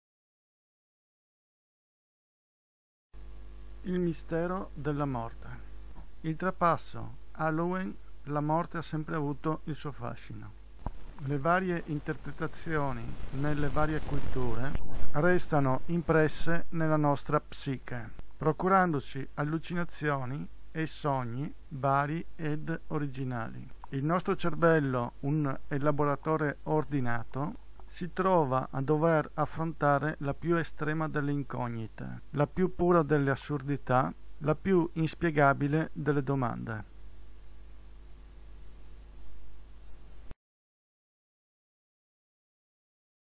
Il mistero della morte
opera incentrata sul trapasso, l'ambiguità del messaggio rivela le possibili interpretazioni nelle diverse culture